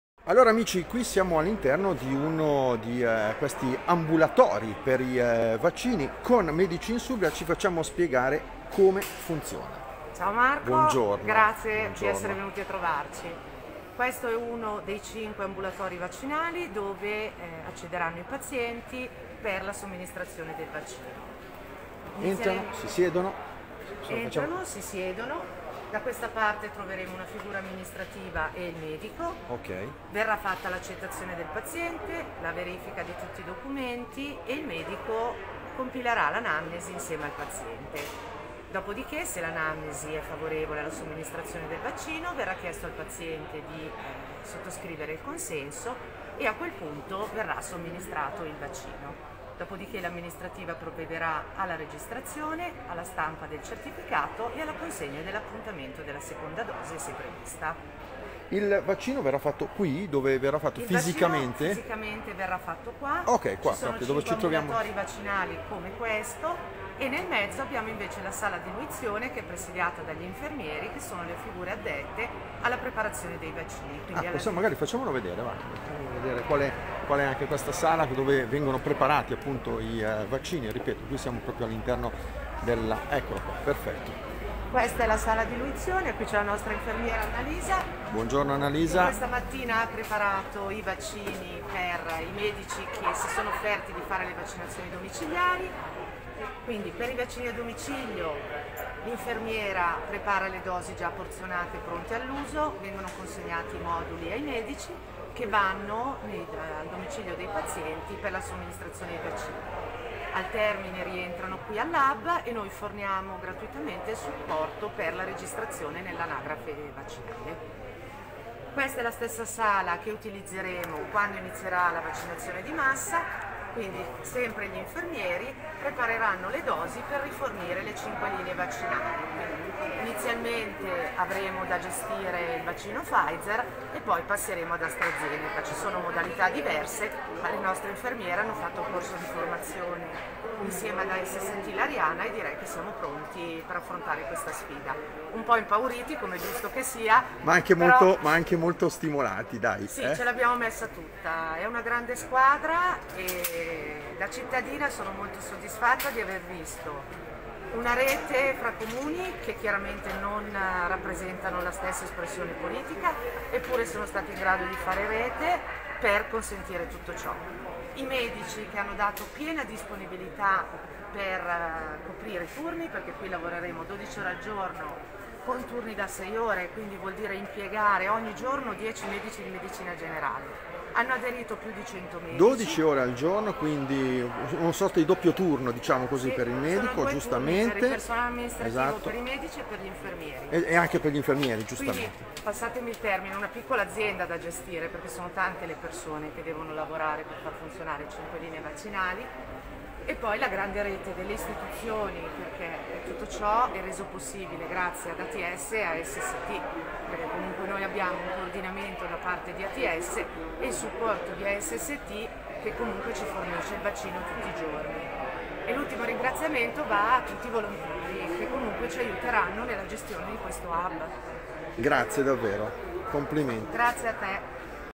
Con Medici Insubria nell'hub vaccinale di Lurate Caccivio
Anteprima CiaoComo: vi facciamo vedere dove e come sarete vaccinati se abitate nell'olgiatese
Simona Clerici di Medici Insubria s.c. ci guida nella struttura di Lurate Caccivio inaugurata poco fa ( vedi precedente post). Qui arriveranno i residenti della zona dell'Olgiatese per la somministrazione. Tutto pronto, dalle pratiche pre-vaccino alle dosi. Un grande impegno, ma ora sì parte davvero.